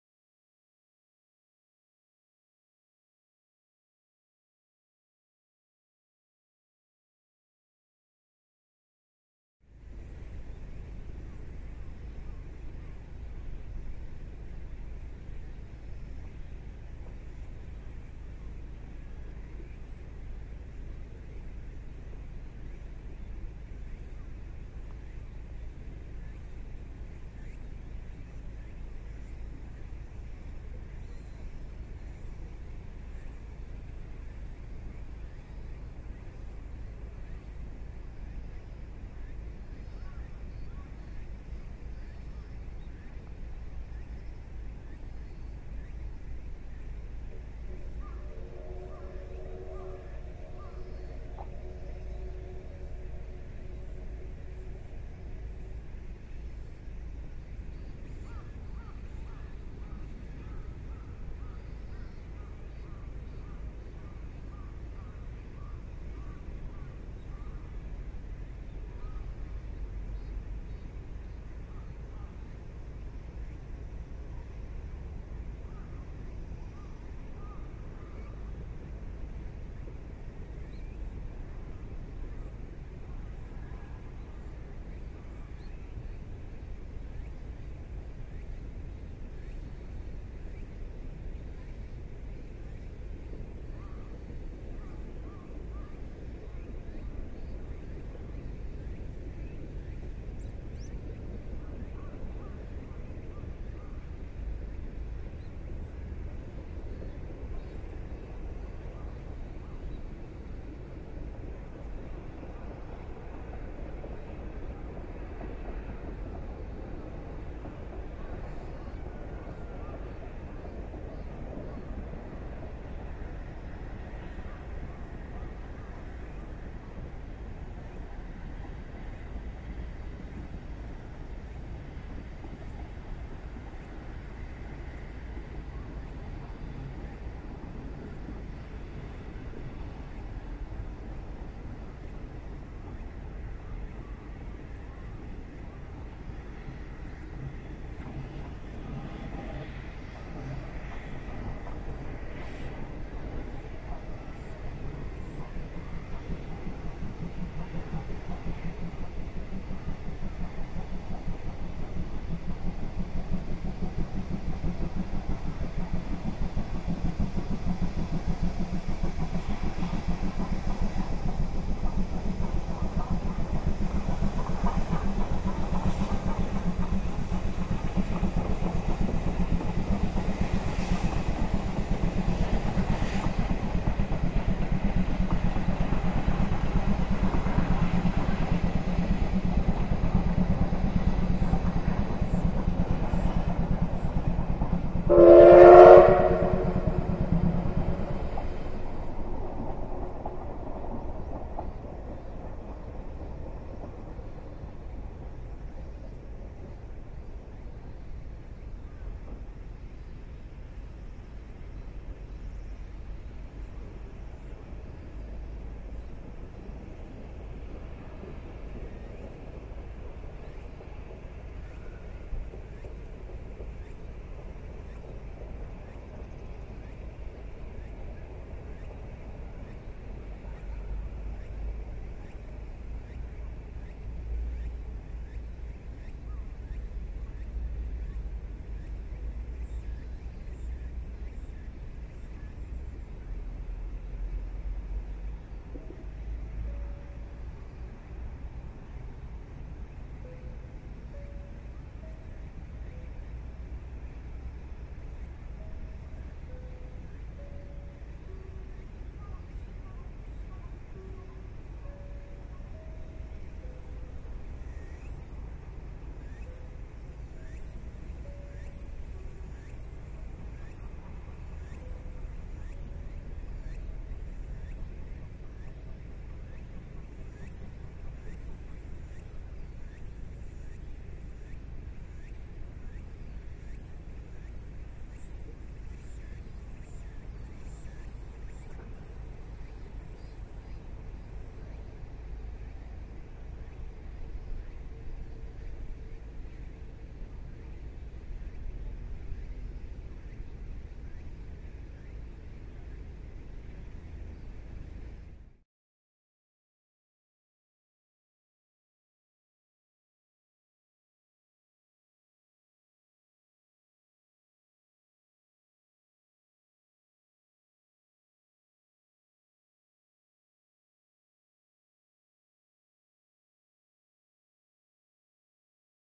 大井川鉄道の蒸気機関車 OIG023 sc 神尾－五和 101ﾚ (C11190)
蒸機急行「かわね路1号」を神尾の対岸の多少下流から収録。この日は C11 190 が牽いていました。勾配にかかるころからブラストがはっきりしてきます。短いトンネルの手前で汽笛が鳴ります。